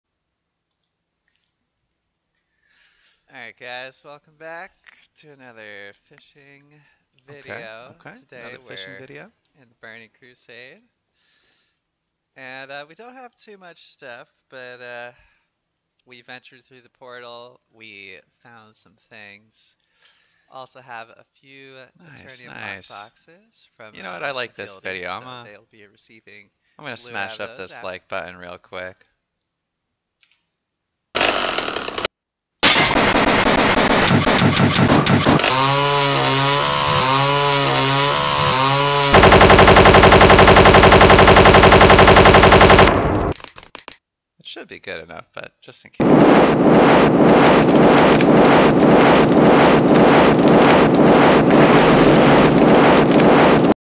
How To Smash That Like Button
A quick smashing the like button tutorial. Hope you enjoy!